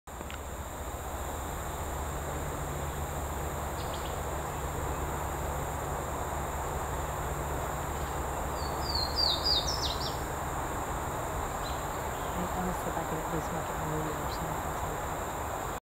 Swainson's Warbling singing, Ocmulgee National Monument
A singing Swainson's warbler on the River Trail at Ocmulgee National Monument, September 1, 2012